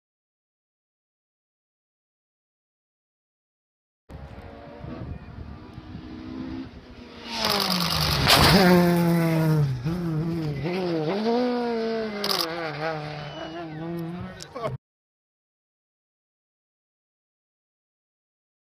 Frank Kelly, Hamiltons Folly Jump 2014
Frank Kelly/Liam Brennan, Massive jump Hamilton's Folly, Circuit of Ireland 2014